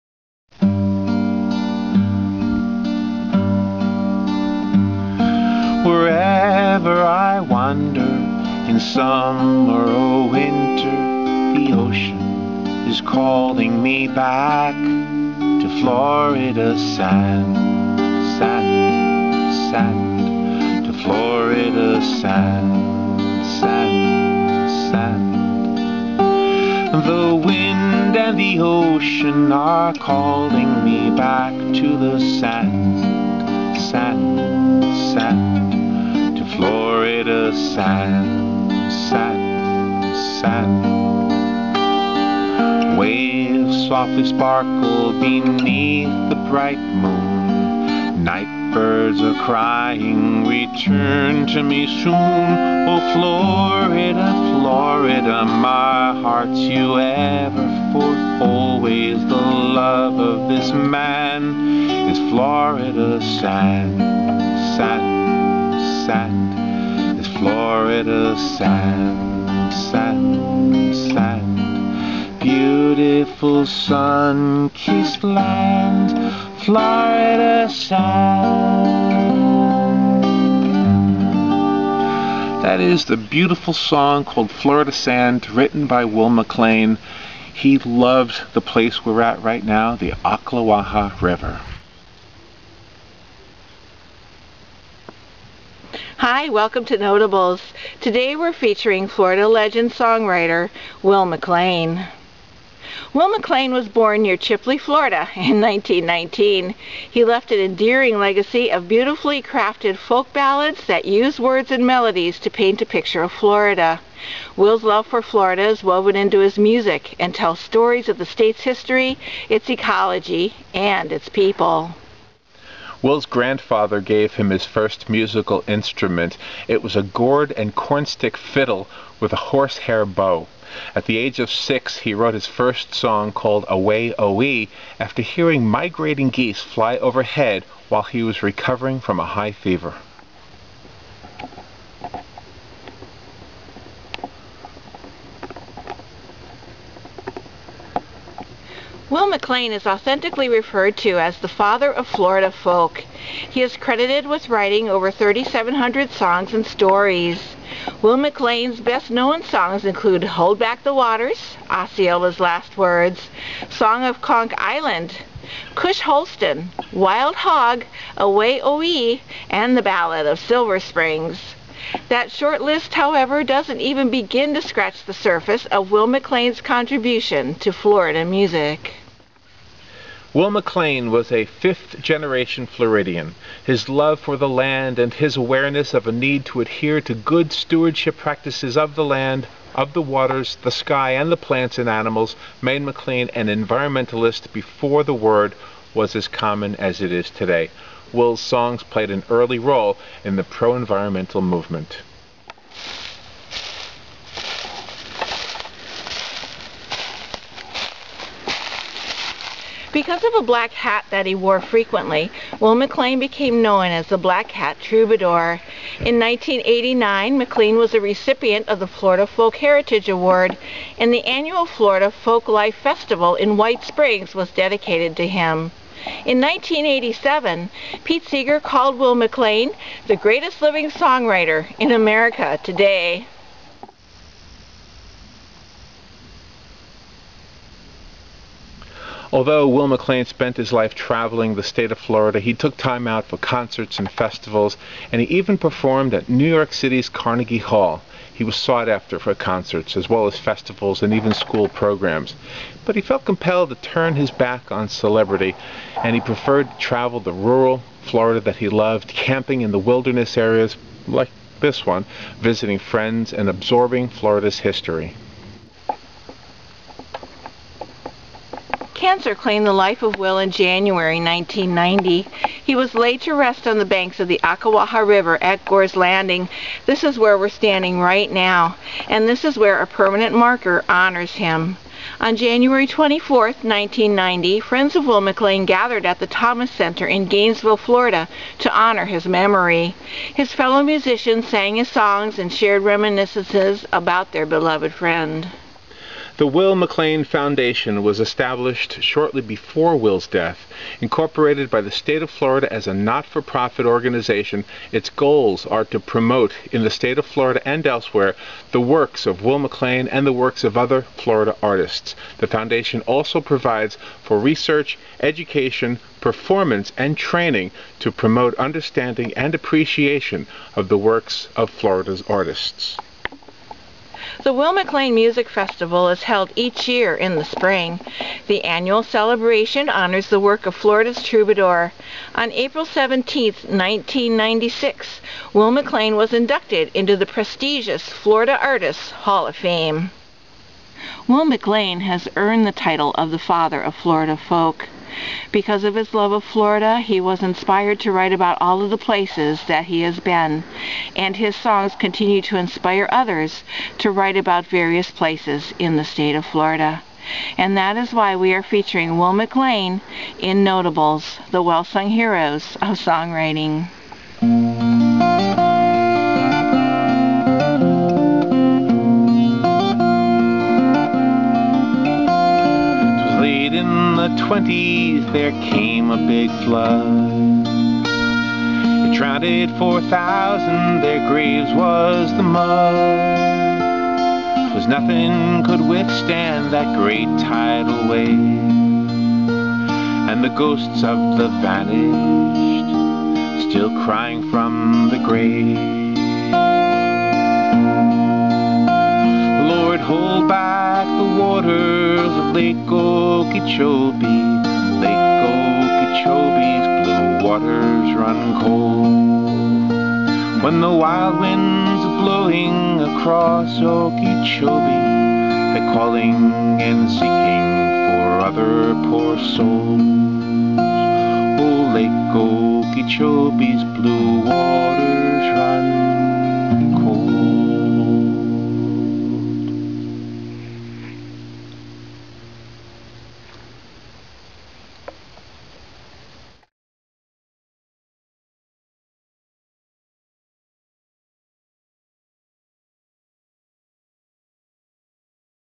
Notables of Songwriting: Will McLean
Singer and Songwriter Will McLean is featured in this installment of
Notables: The Well Sung Heroes Of Songwriting with Robin And The Giant.